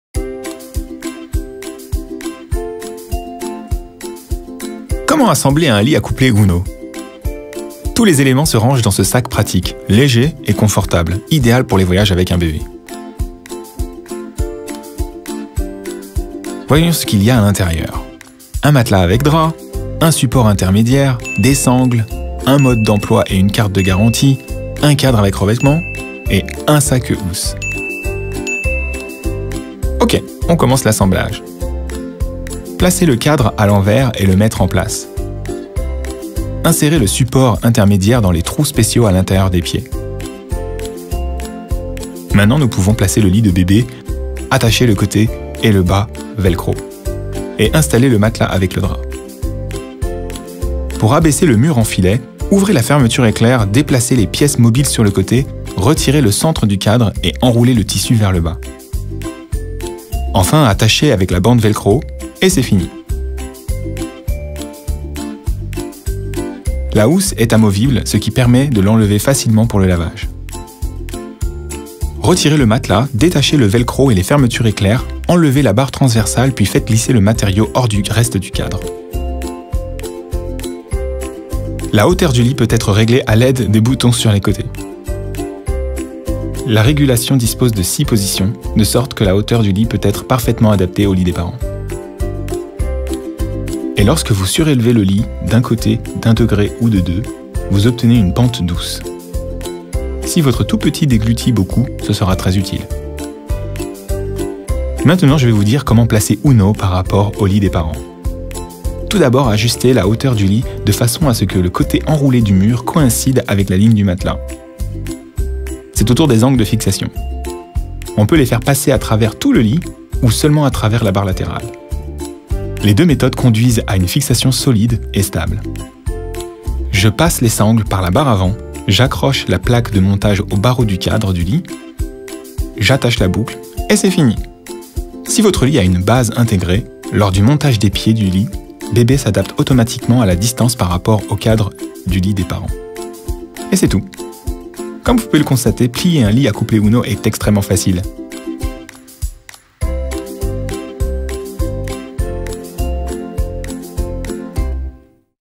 Guide de montage et d'utilisation : Cododo UNO de kinderkraft
Assemblez votre berceau cododo en un rien de temps grâce à ce guide qui vous permettra d'installer bébé rapidement.
👉 Besoin d'aide et de conseils ? Notre équipe se tient à votre disposition